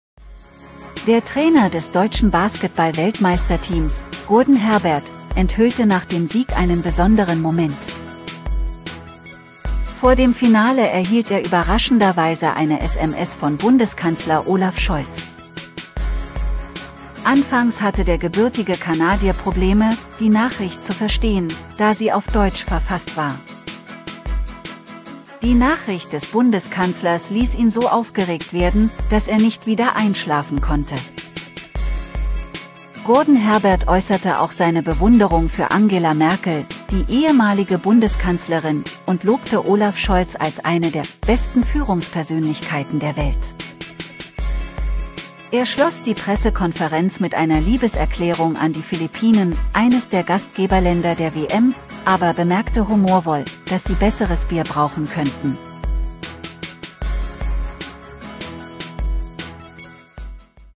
So raubte Scholz dem Basketball-Nationaltrainer den Schlaf
Der Trainer des deutschen Basketball-Weltmeisterteams, Gordon Herbert, enthüllte nach dem Sieg einen besonderen Moment. Vor dem Finale erhielt er überraschenderweise eine SMS von Bundeskanzler Olaf Scholz.